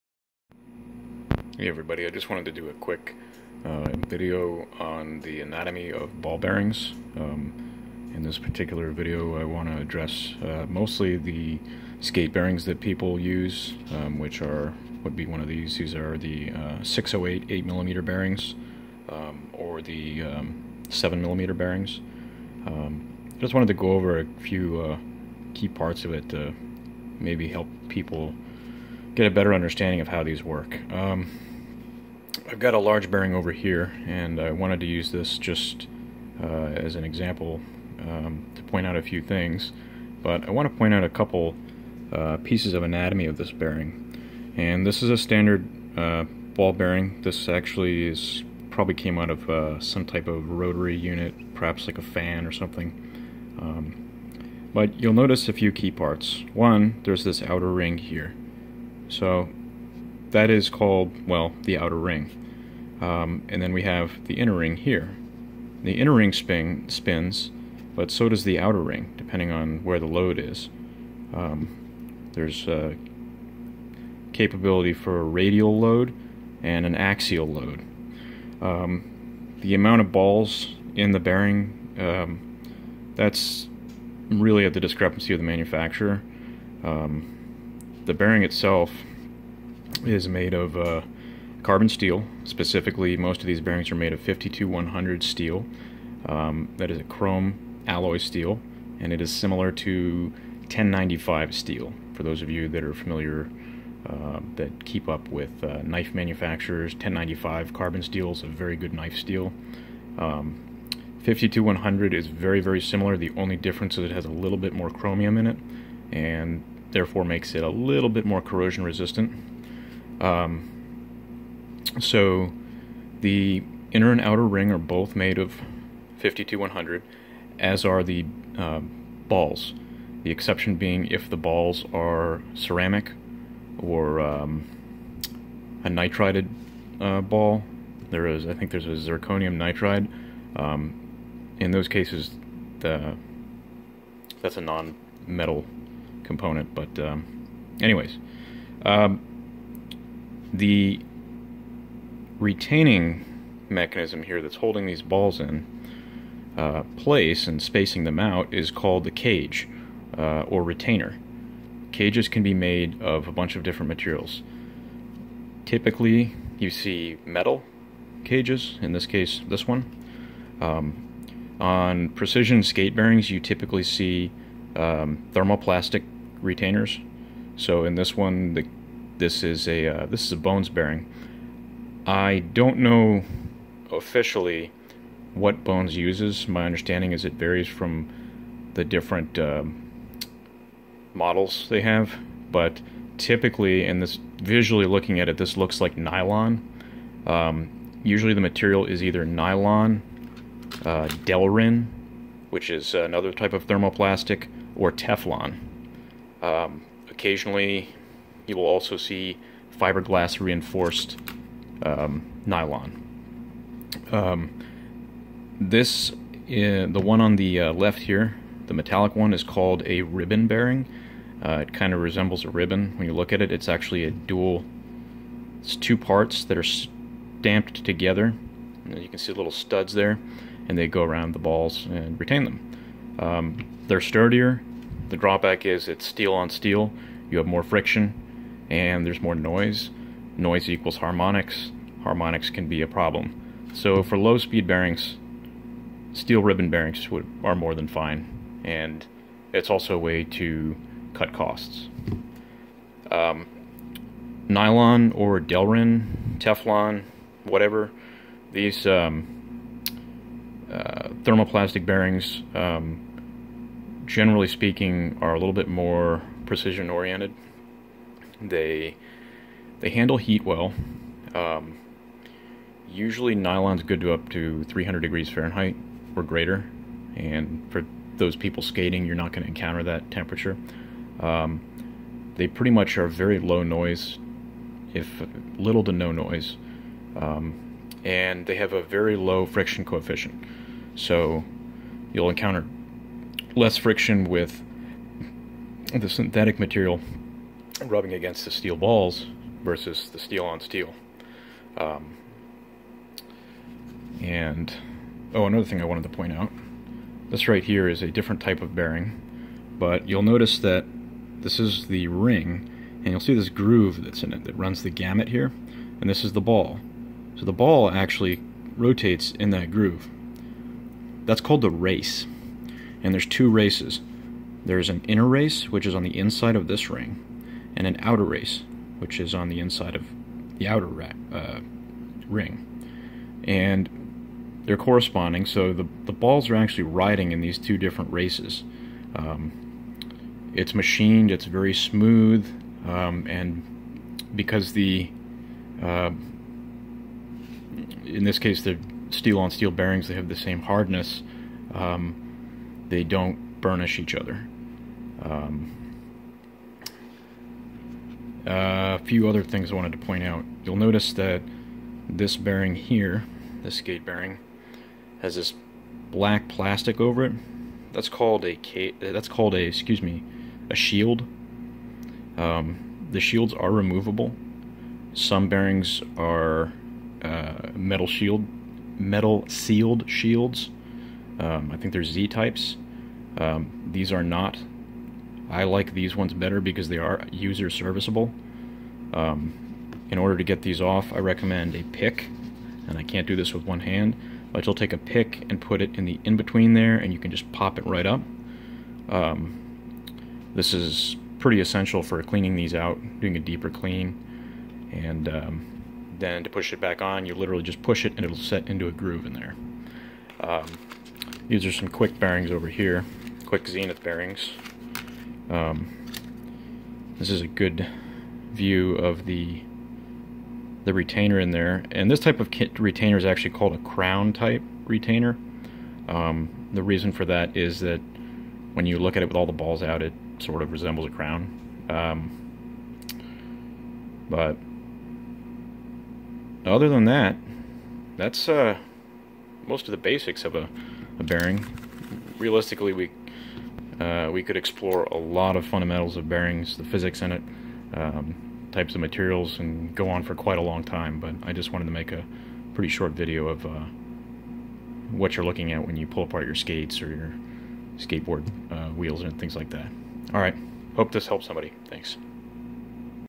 Ball Bearings Anatomy
A semi brief look at ball bearings their materials and components.